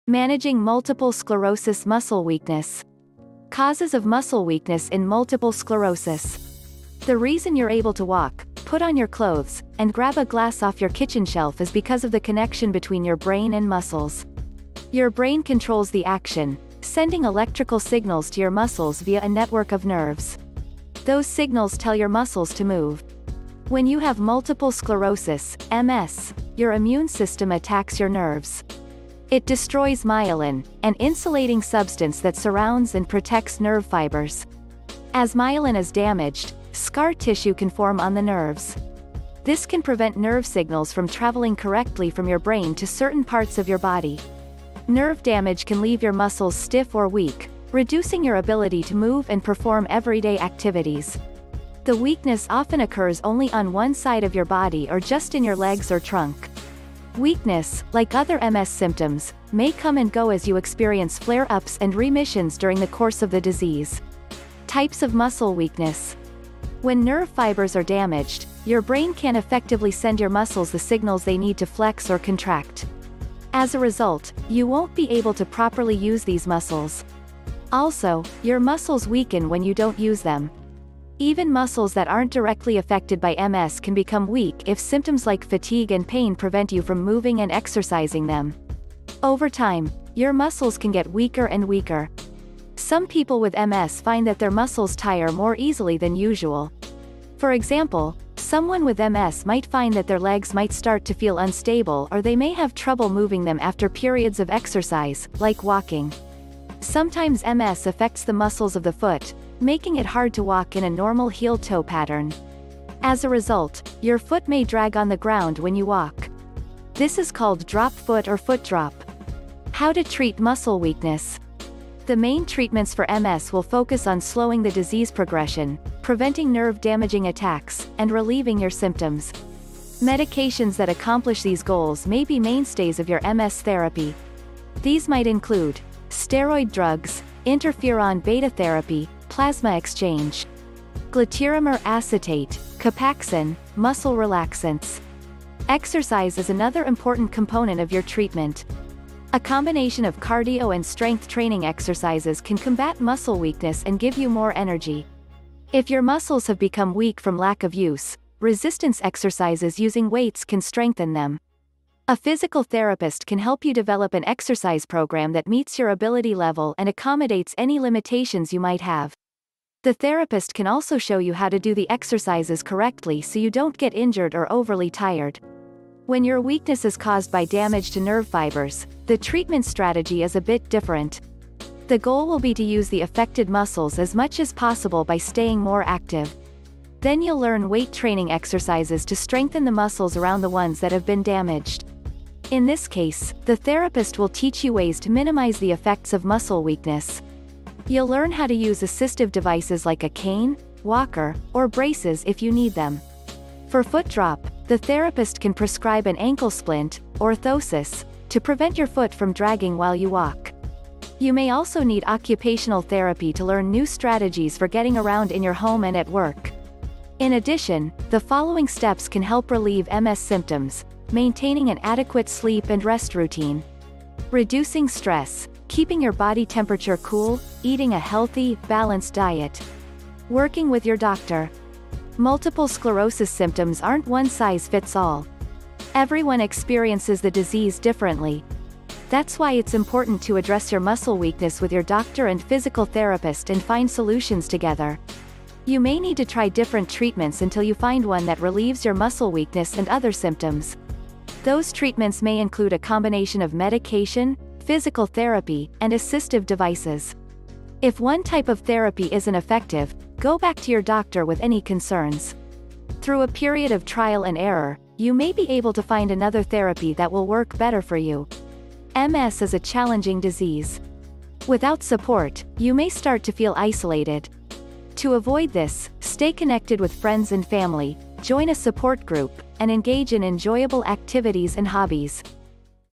Managing Multiple Sclerosis Muscle Weakness
Causes of muscle weakness in multiple sclerosis.
The reason you’re able to walk, put on your clothes, and grab a glass off your kitchen shelf is because of the connection between your brain and muscles. Your brain controls the action, sending electrical signals to your muscles via a network of nerves. Those signals tell your muscles to move.
When you have multiple sclerosis (MS), your immune system attacks your nerves. It destroys myelin, an insulating substance that surrounds and protects nerve fibers.
As myelin is damaged, scar tissue can form on the nerves. This can prevent nerve signals from traveling correctly from your brain to certain parts of your body.
Nerve damage can leave your muscles stiff or weak, reducing your ability to move and perform everyday activities. The weakness often occurs only on one side of your body or just in your legs or trunk.
Weakness, like other MS symptoms, may come and go as you experience flare-ups and remissions during the course of the disease.